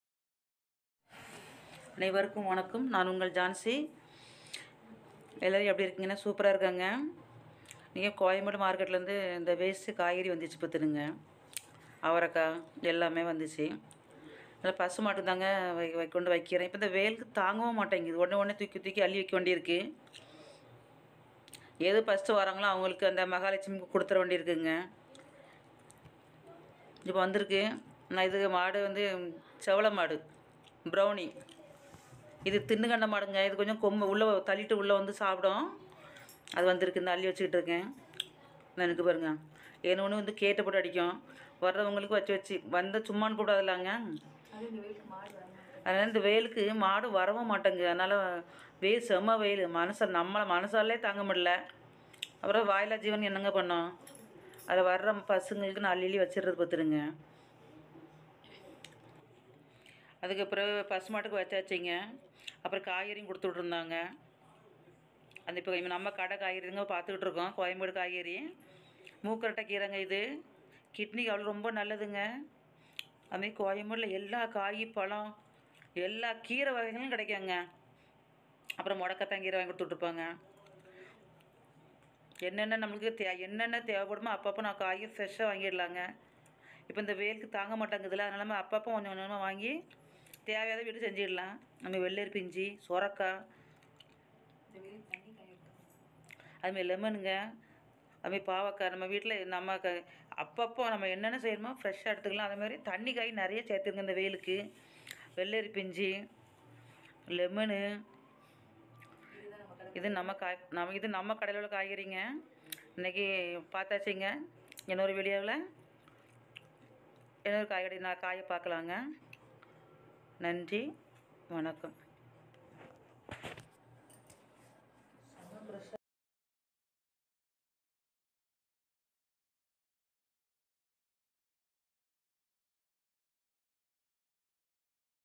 நம்ம வீட்டுக்கு வர மகாலட்சுமிக்கு அவரை தீவனம் அளித்தல் 🤩எங்க கடை காய்கறிகள் 🤩 👇வாங்க பார்க்கலாம் ❤️
நம்ம வீட்டுக்கு வர மகாலட்சுமிக்கு செவ்வழை   கோமாதா அவரை தீவனம் அளித்தல் 🤩🥰எங்க கடை காய்கறிகள் 🤩 குறைந்த விலை 👇வாங்க பார்க்கலாம் ❤️ Fresh load koyembedu  Market vegetables, fruit and flowers 😍 with low cost selling directly from koyembedu market for marriage event @jancysenthilvlog2680

In this video I share about brown cow visiting my house feeding food and water Unboxing  my our shop vegetables 🙏💖😍

அனைவருக்கும் வணக்கம் நான் உங்கள் ஜான்சி (திருநெல்வேலி )Chennai Vlog.